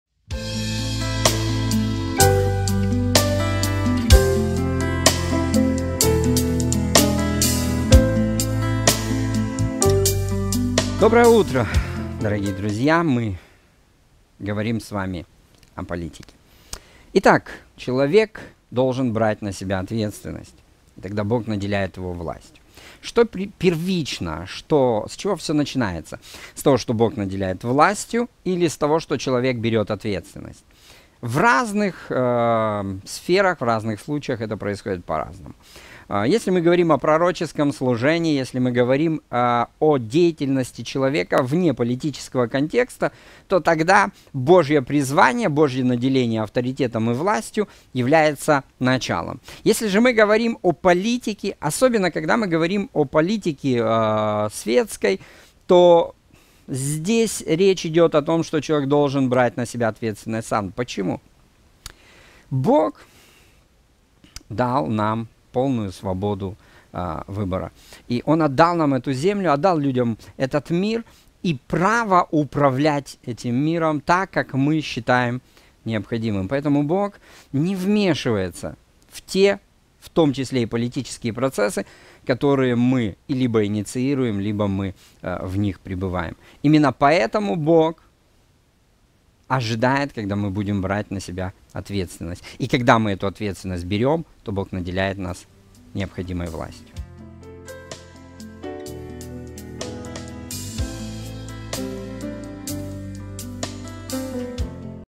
ЧТО ПЕРВИЧНО? ВЛАСТЬ ИЛИ ОТВЕТСТВЕННОСТЬ?
Человек берет на себя ответственность и таким образом получает власть? Или же Бог дает власть и вместе с ней приходит ответственность?